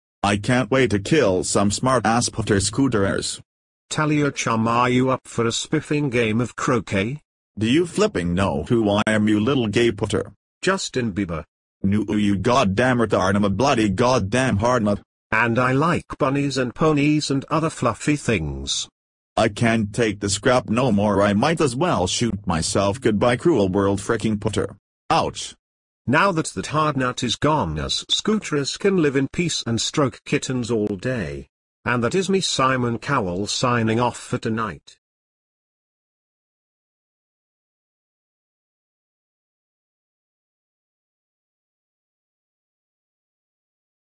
hardnut scooter killer vs simon cowell episode 1
Simon Cowell pays a visit to the hardnut but when the hardnut is not interested in partaking in croquet they embark in the fiercest rivalry in History based on the true story which inspired the movie Mrs Browns boys D'movie and inception, this tale will have you glued to your seats